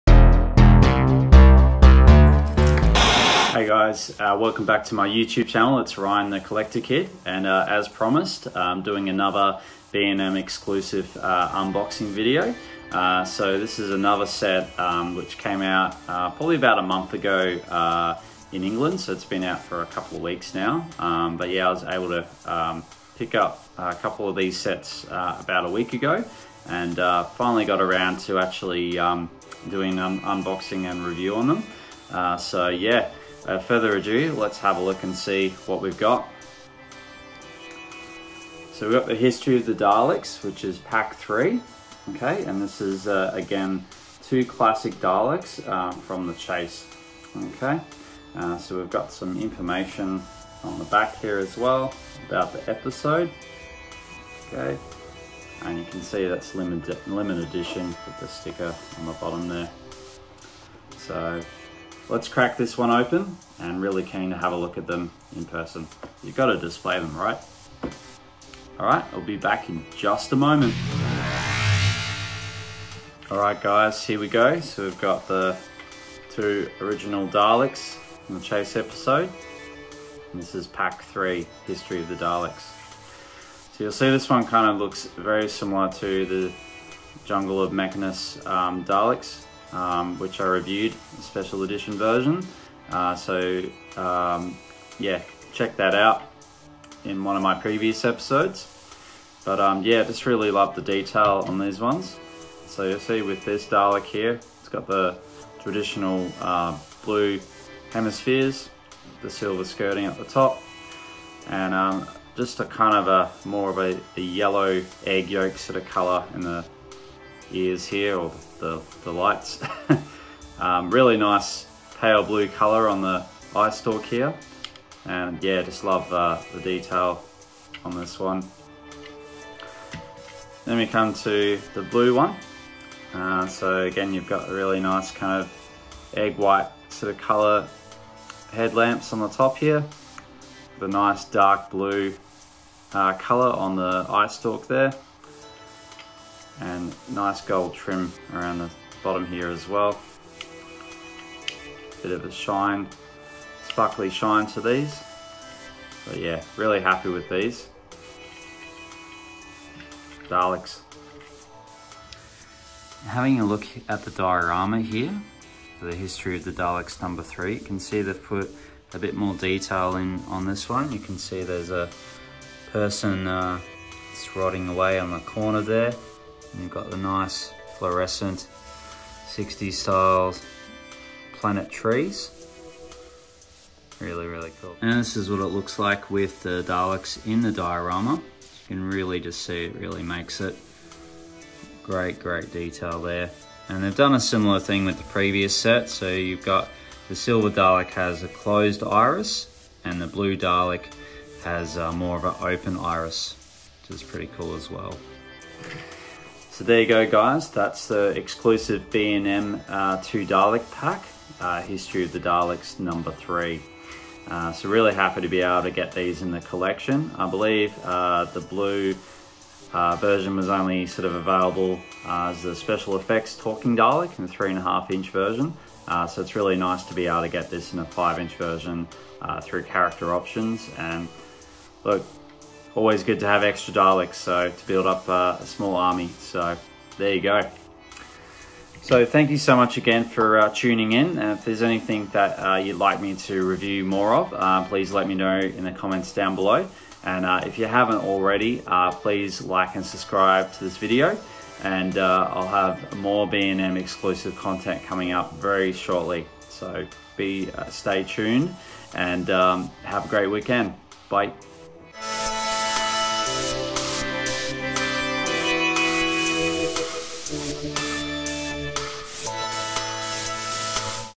The Collector Kid Episode 23 B&M History of the Daleks #3 Unboxing Review
In this episode of the collector kid we look at the B&M Exclusive History of the Daleks set #3 in a unboxing review.